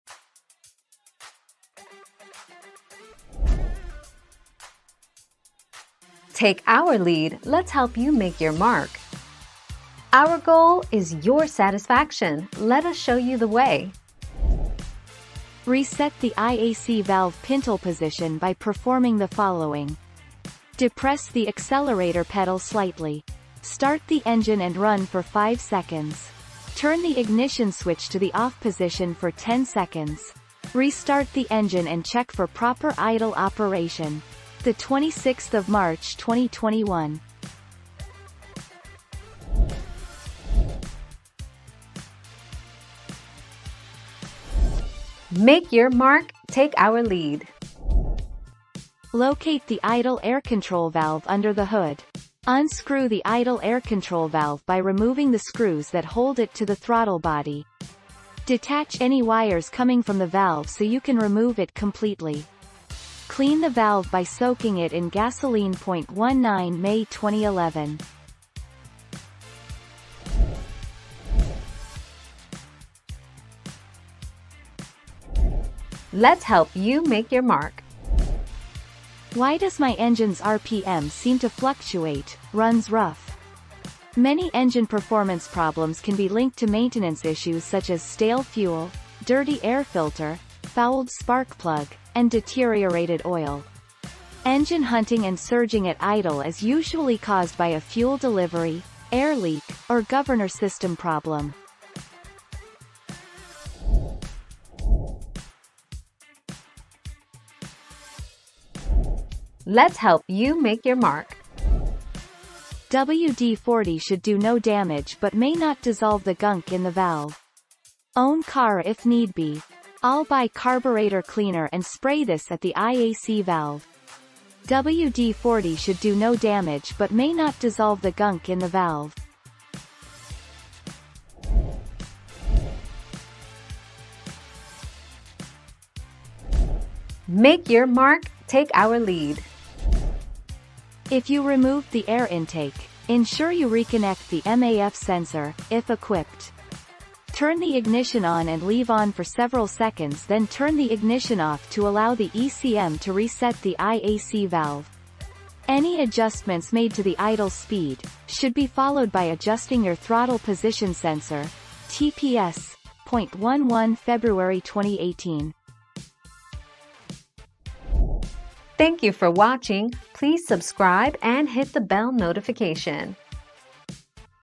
How do I reset my idle air control valve?
00:00 - How do I reset my idle air control valve?
00:41 - Can you clean an idle air control valve?
01:12 - What causes RPM to fluctuate while idling?
01:45 - Can I use WD40 to clean IAC valve?
02:12 - Can you adjust IAC valve?

Laura S. Harris (2021, April 18.) How do I reset my idle air control valve?
    AskAbout.video/articles/How-do-I-reset-my-idle-air-control-valve-237452